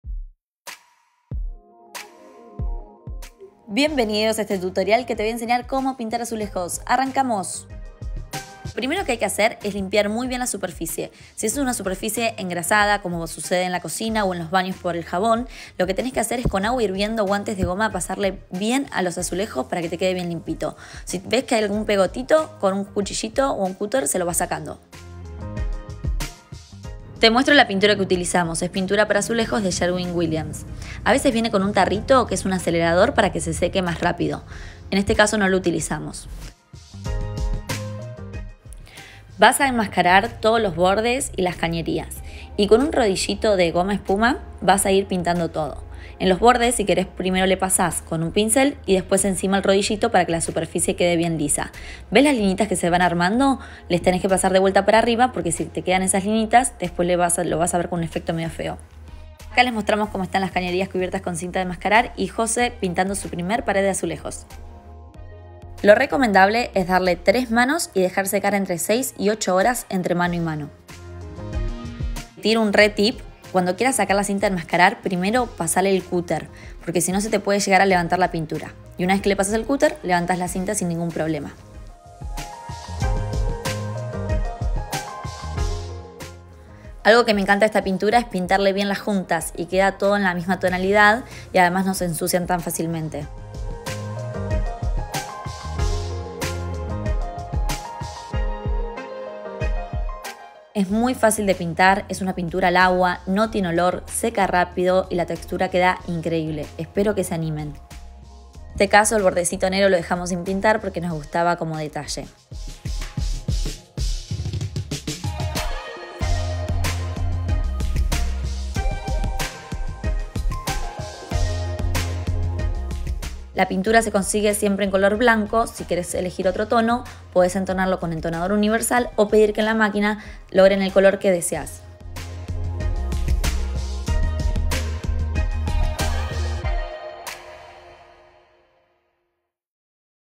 Tutorial para pintar azulejos de cocina o baño
1) limpiar bien con agua hirviendo y una rejilla limpia . Si se salta es por que estaba engrasada la superficie
2) enmascarar bordes caños y las canillas podes desenroscar si están en la pared .
3) Usamos pintura para azulejos de @sherwinarg  y rodillo de espuma y un pincel
4) dejamos secar entre mano y mano 6 hs . Dimos 3 manos . Retiramos con cúter la cinta de enmascarar
LISTO ! Si pintas el baño te recomiendo no usar la ducha por 48 hs para que seque bien .
Usamos un tarro de un litro para toda la cocina y ronda entre 1400 y 1100 pesos el litro
Espero que me manden sus antes y después

Para seguir aprendiendo de decoración y reforma seguiré en Instagram @decopordosmangos